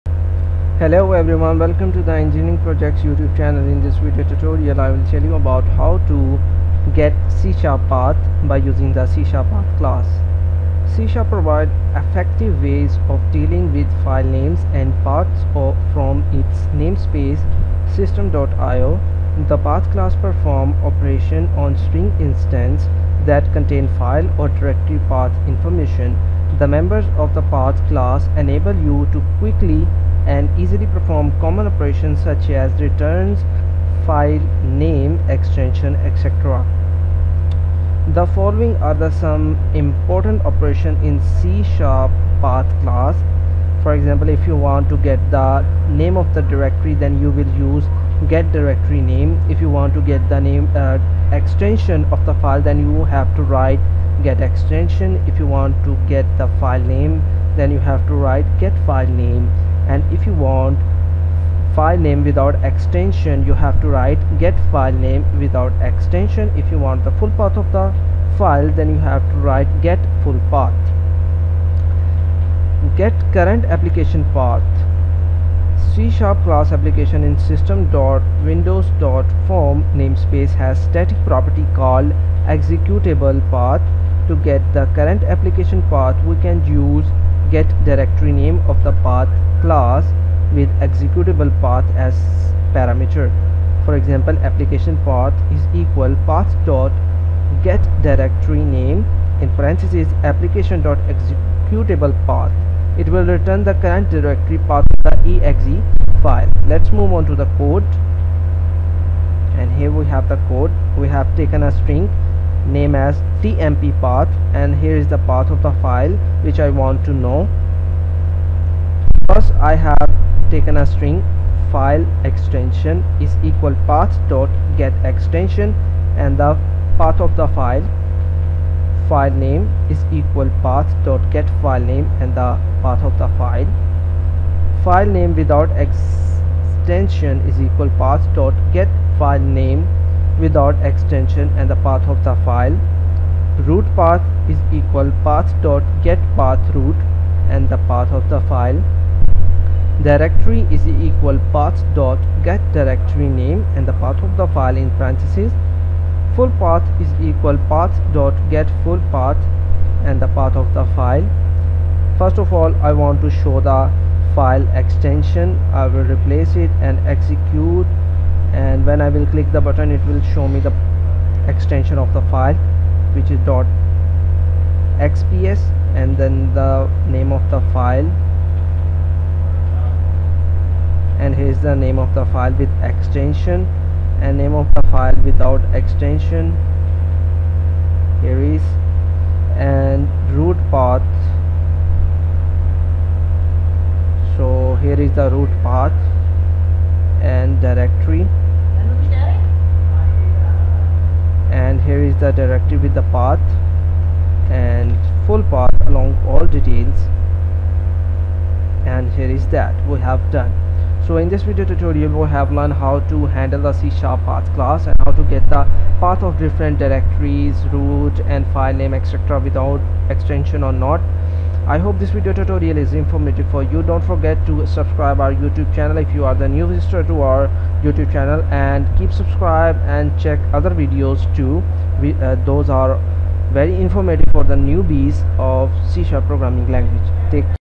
092 - How to C# Path Class
C# provides effective ways of dealing with filenames and paths from its namespace System.IO . The Path Class performs operations on String instances that contain file or directory path information. The members of the Path class enable you to quickly and easily perform common operations such as returns filenae , extensios etc.

The following are the some important operations in C# Path Class:

GetDirectoryName - Returns the directory information for the specified path string.

GetExtension - Returns the extension of the specified path string.

GetFileName - Returns the file name and extension of the specified path string.

GetFileNameWithoutExtension - Returns the file name of the specified path string without the extension.

GetFullPath - Returns the absolute path for the specified path string.

Get Current Application Path

C# Class application in System.Windows.Forms namespace has static property called ExecutablePath . To get the current application path we can use GetDirectoryName of Path class with ExecutablePath as parameter.

  applicationPath = Path.GetDirectoryName(Application.ExecutablePath)
It will return the current directory path of the .exe file .